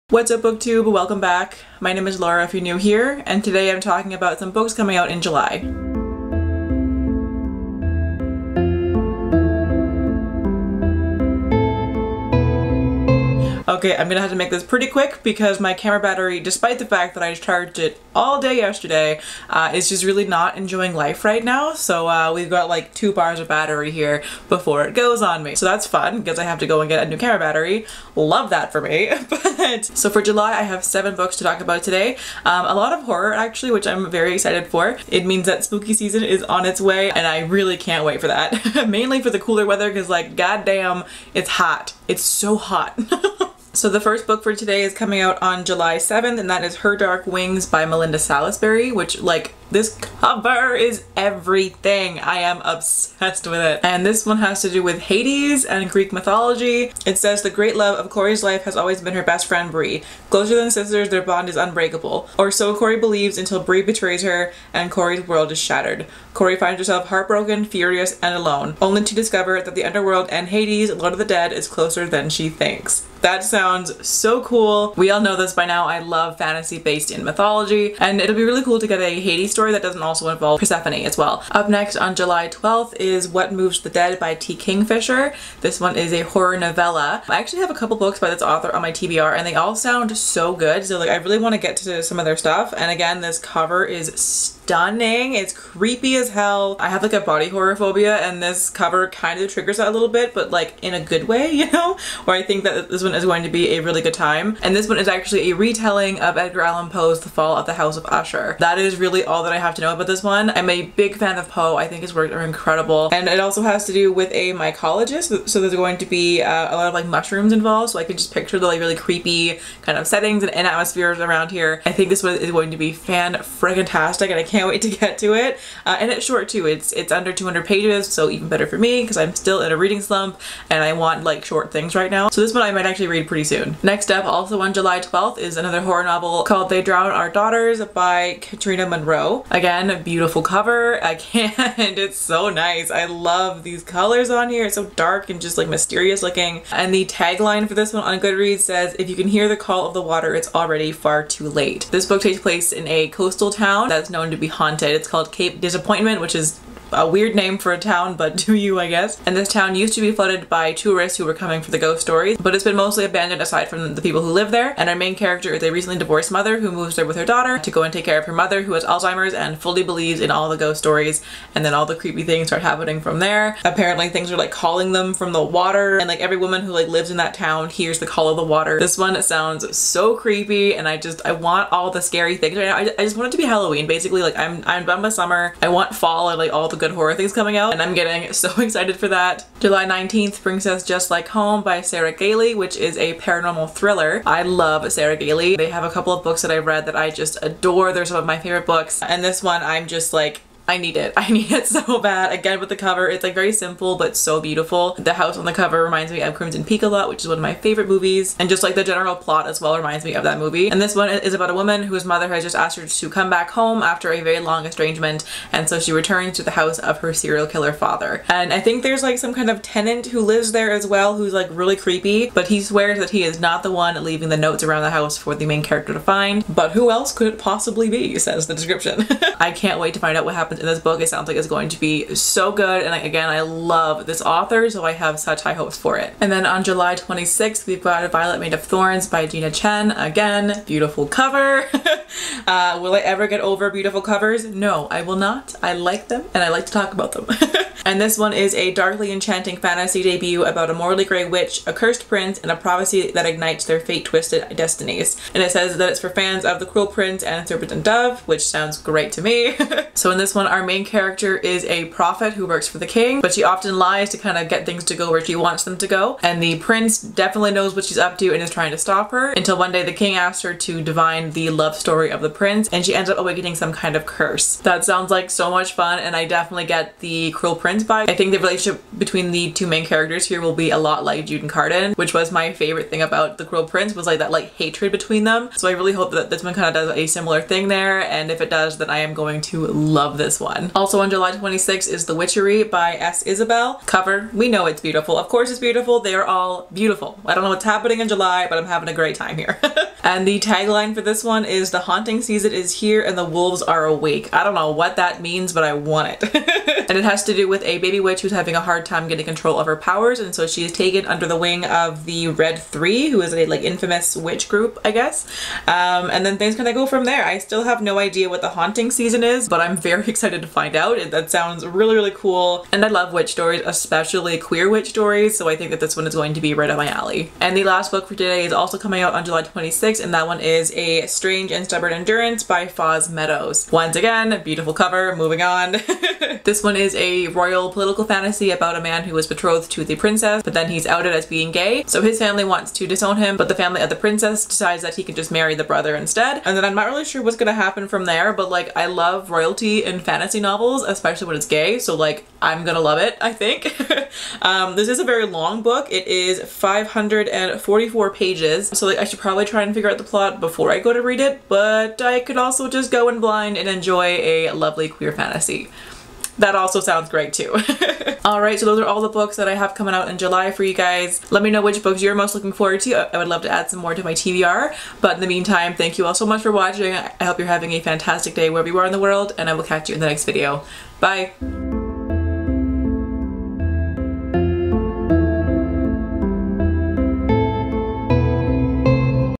New Book Releases for July 2022
They/them

You ever film a video and then completely forget it exists? This was filmed almost a month ago lmfao my bad

★Books mentioned:
☆Her Dark Wings by Melinda Salisbury
☆What Moves the Dead by T. Kingfisher
☆They Drown Our Daughters by Katrina Monroe
☆Just Like Home by Sarah Gailey
☆Violet Made of Thorns by Gina Chen
☆The Witchery by S. Isabelle
☆A Strange and Stubborn Endurance by Foz Meadows

★My links:
☆Instagram - @me.myshelf.andi (best place to chat!)